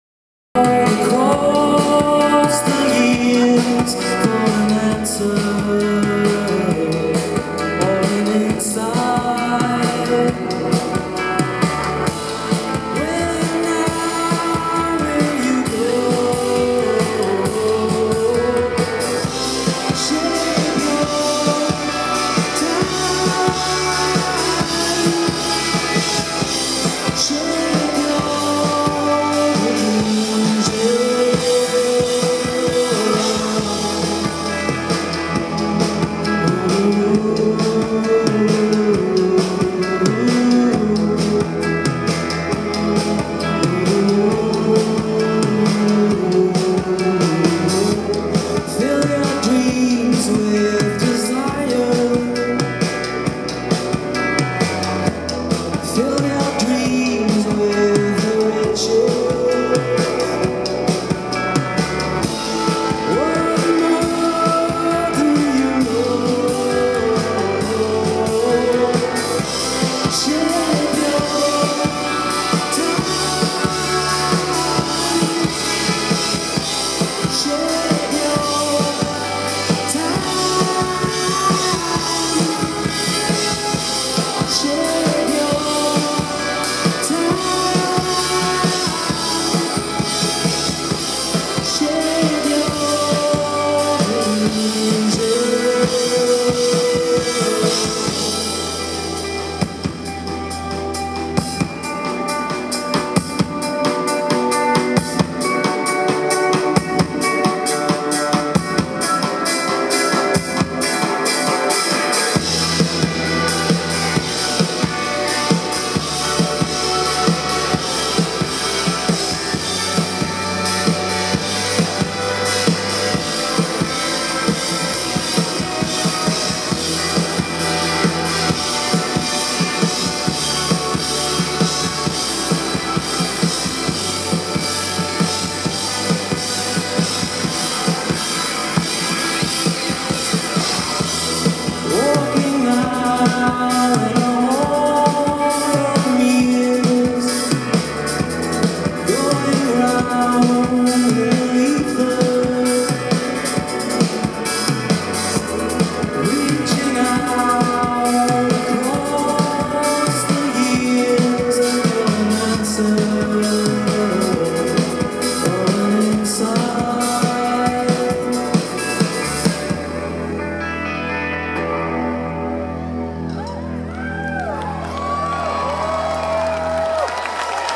Helio Sequence new song
Helio Sequence live at Bumbershoot 2012.  Song off new album